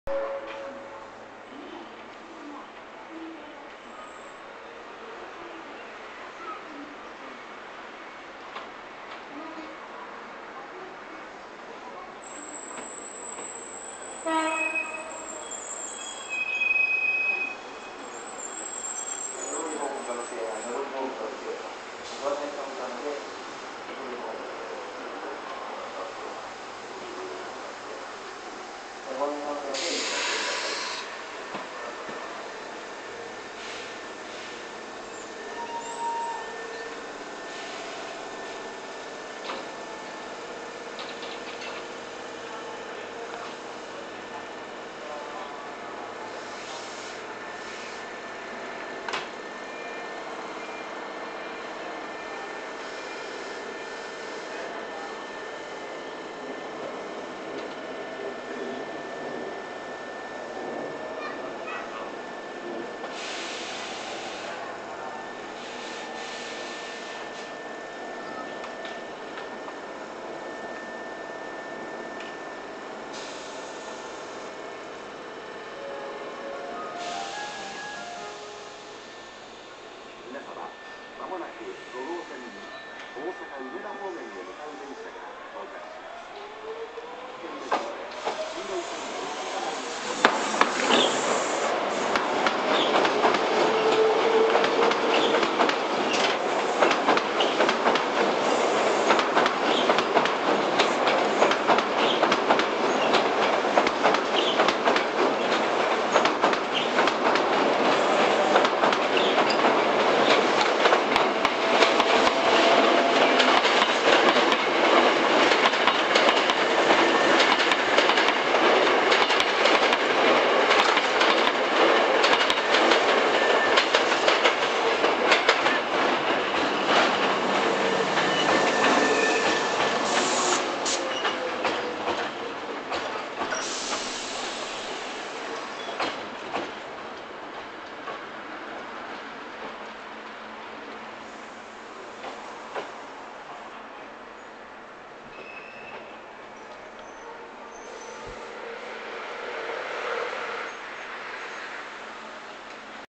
阪急電鉄 5300系 5321F 普通 北千里行き 大阪市営地下鉄 66系 普通 高槻市行き 淡路駅にて 淡路駅平面交差
阪急電鉄
5300系 5321F 普通 北千里行き
大阪市営地下鉄 66系 普通 高槻市行き
淡路駅にて 淡路駅平面交差